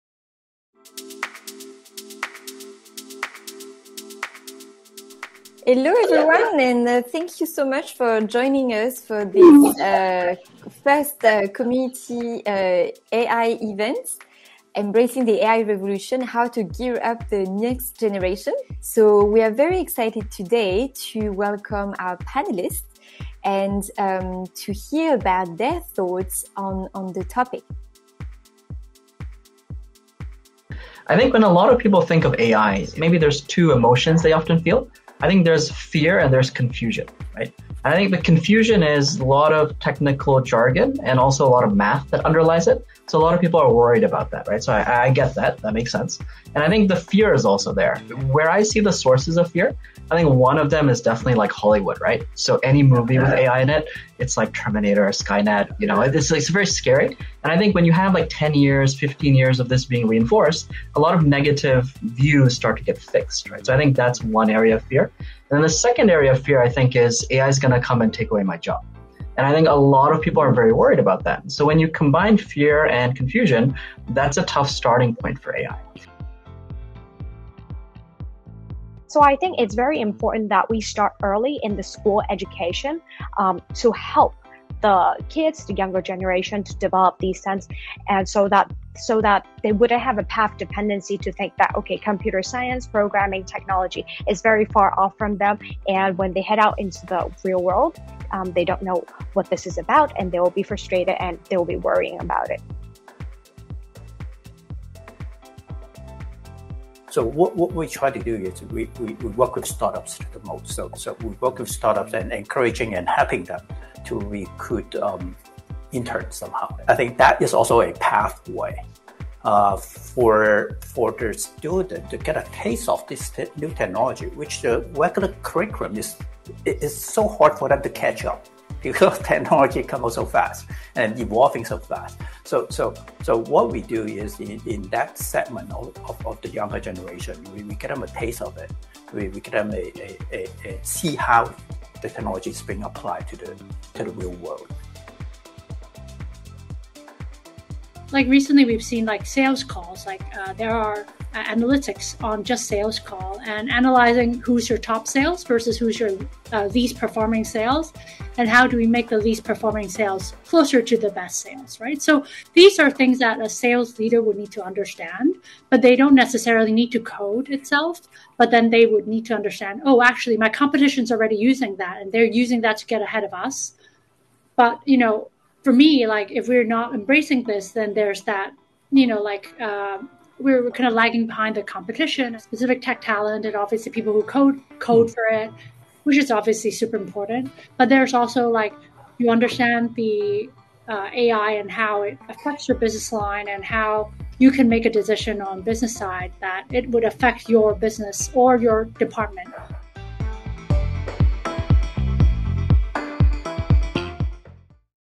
[直播精華|Highlights]擁抱人工智能:如何為下一代做好準備？Embracing the AI Revolution: How to gear up the next generation?
In our webinar "Embracing the AI Revolution: How to gear up the next generation?" on 7 March 2022, we reviewed the current development of the AI community in Hong Kong as well as the importance of AI education. Curious about the key takeaways from the panel discussion? Check out the summary here!

在3月7日的網上研討會「擁抱人工智能：如何為下一代做好準備？」中，我們除了討論到香港現時人工智能的發展情況，也了解到人工智能教育的重要性。想知道當天的重點嗎？按此處瀏覽文章！